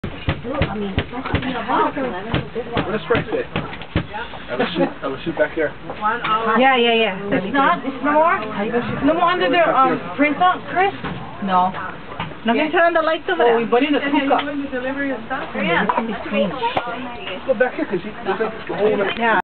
chris and grant cubicle hunting for the spider
there was a spider in the office. two co-workers search for it in the cublicles...